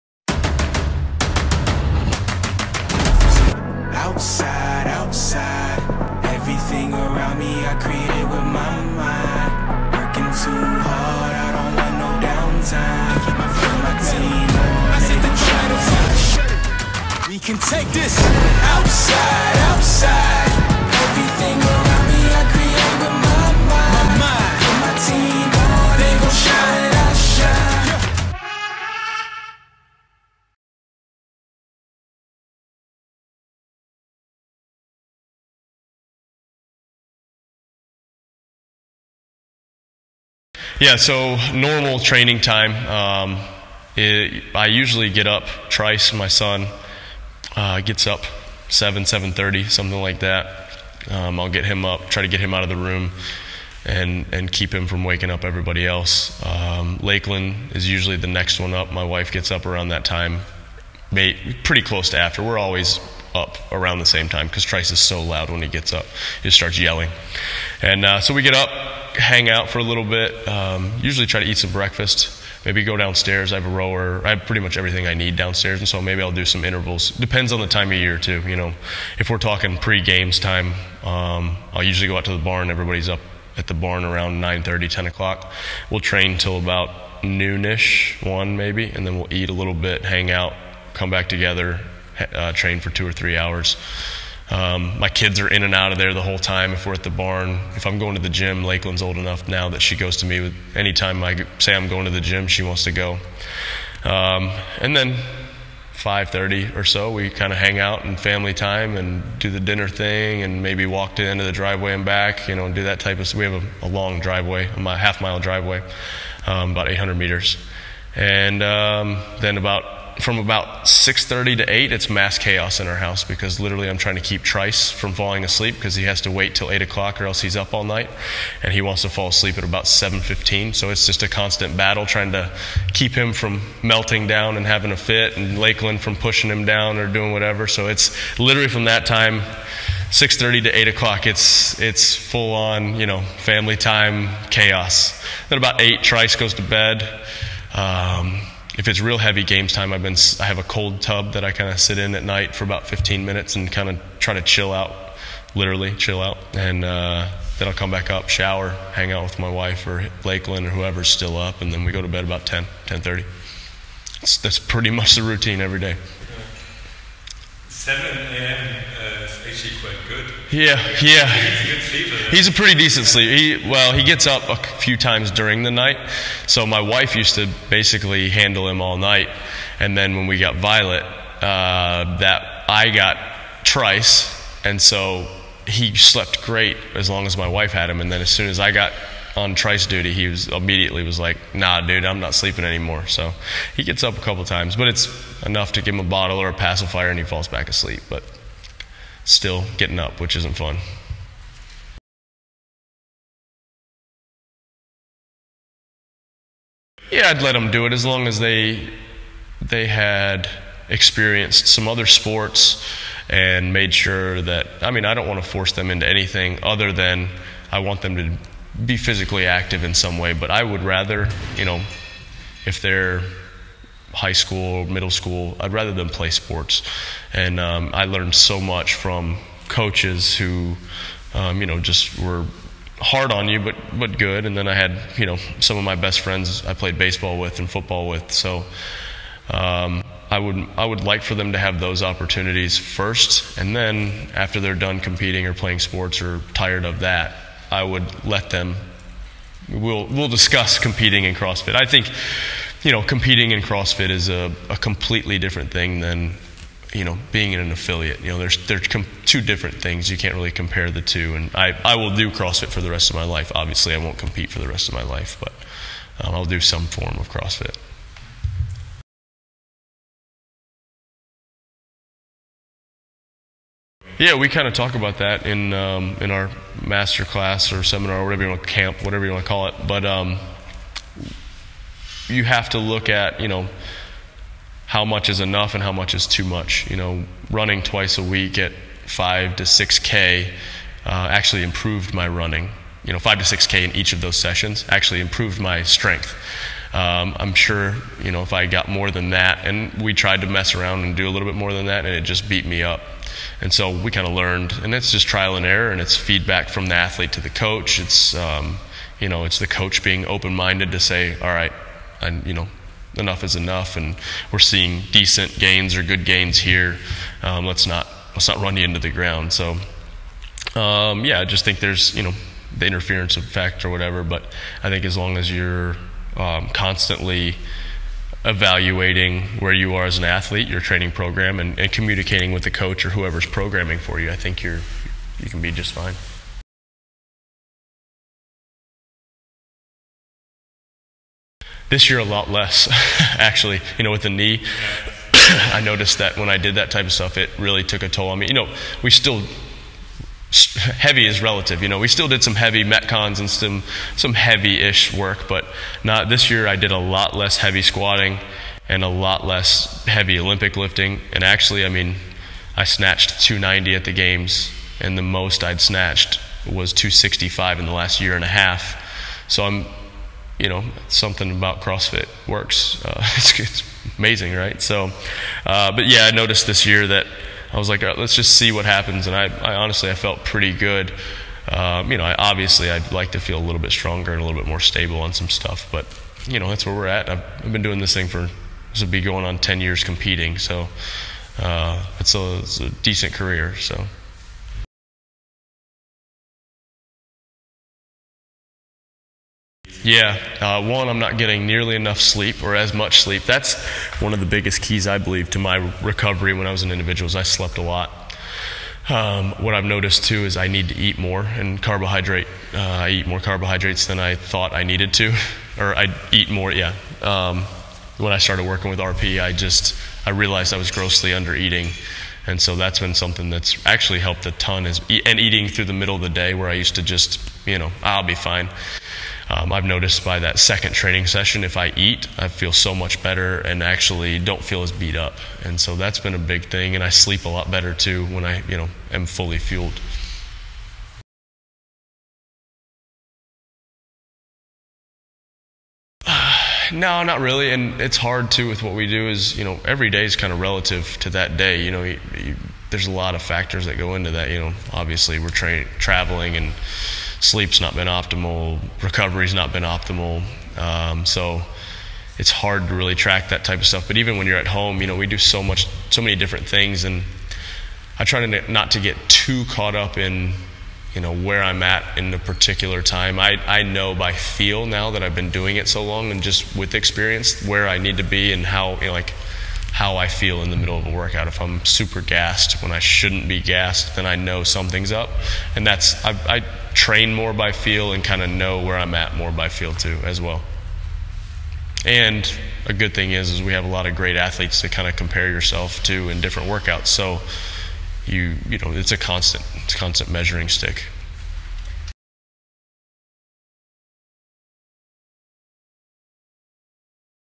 Rich Interview Part 4 Training & Programming
Rich Froning spoke to BOXROX about his training and programming.